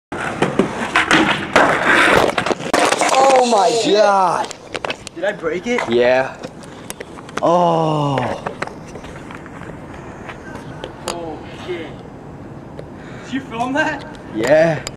fisheye lens brake
smashed the front of my camera off